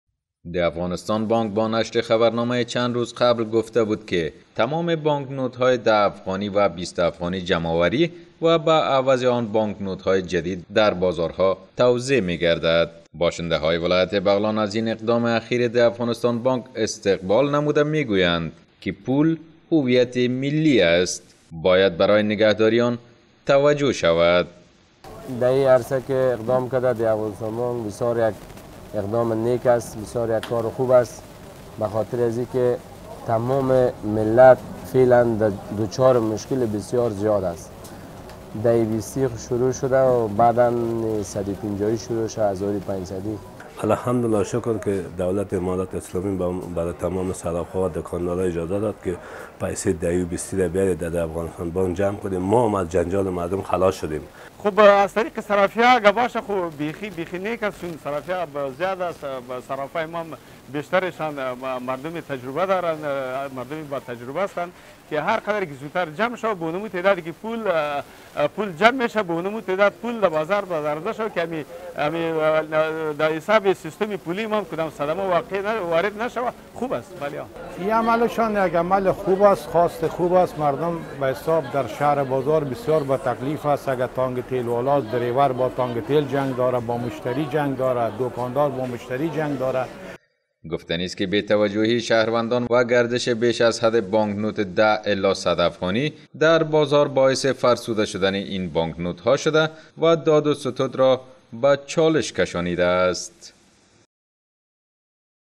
بانک‌‌ نوت‌های 10 و 20 افغانی مندرس شده جمع‌آوری و در بدل آن بانک‌نوت‌های جدید توزیع می‌شود
باشندگان بغلان از تصمیم بانک مرکزی کشور که بر اساس آن بانک‌‌نوت‌های ۱۰ و ۲۰ افغانی مندرس شده جمع‌آوری و در بدل آن بانک‌نوت‌های جدید توزیع می‌شود، استقبال می‌کنند